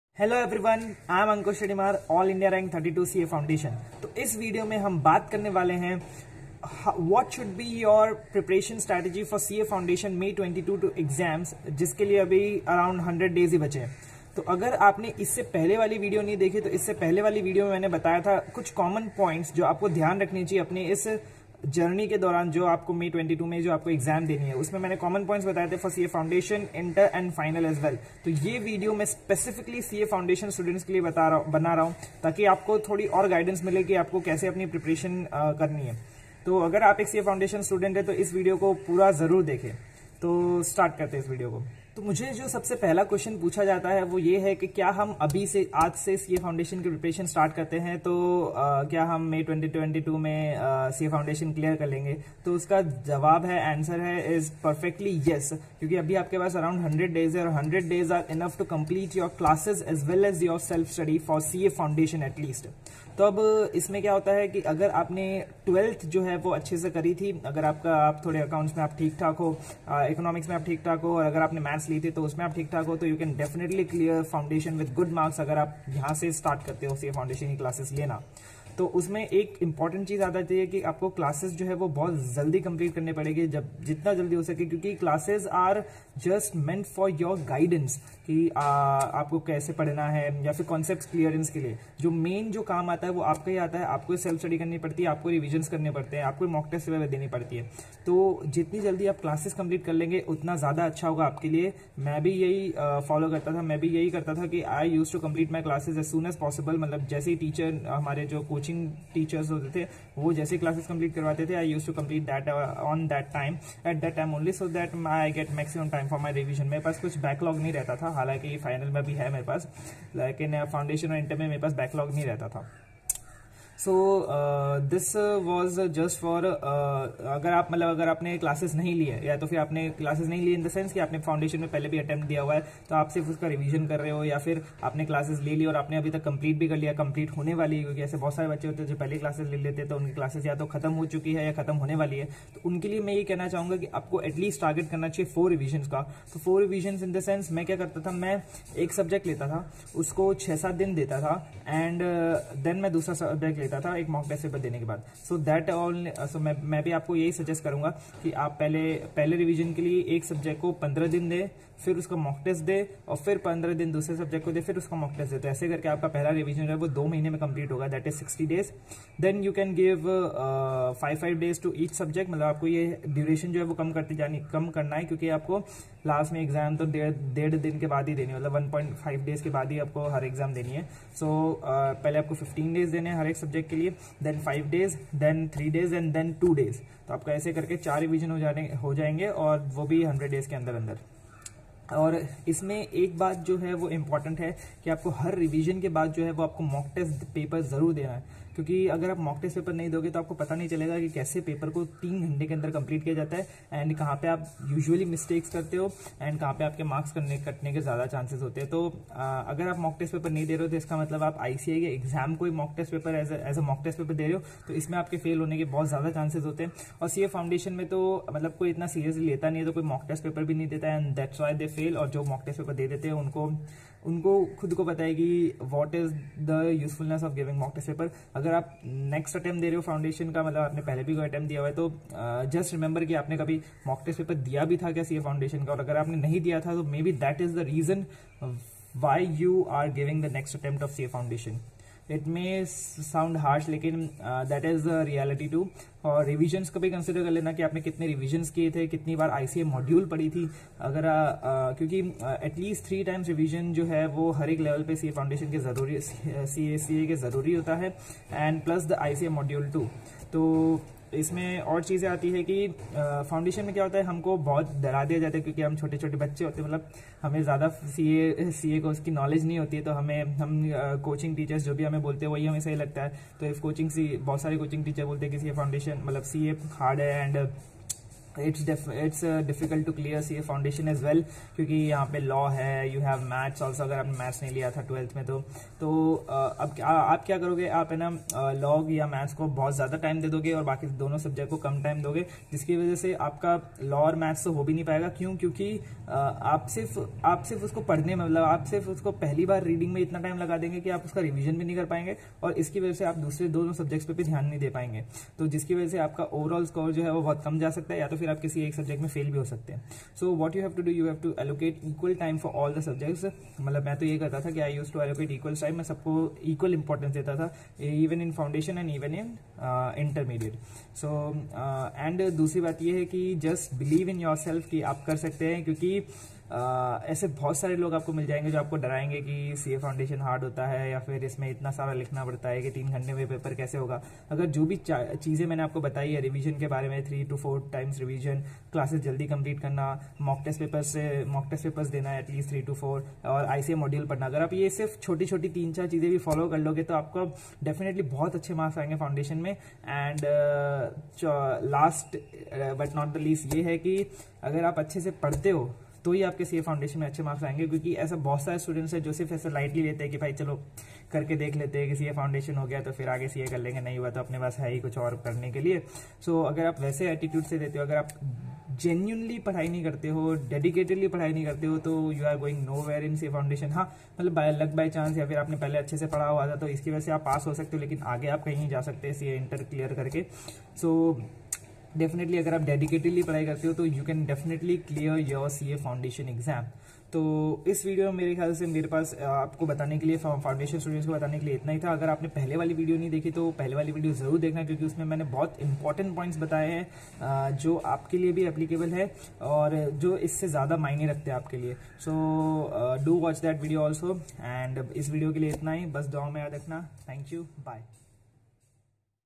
CA Foundation May 2022 | Plan your way Forward! | AIR 32 CA Foundation
Since the exams dates are announced, I have shared my strategy for the last 4 months you have! Hope you find it valuable!!!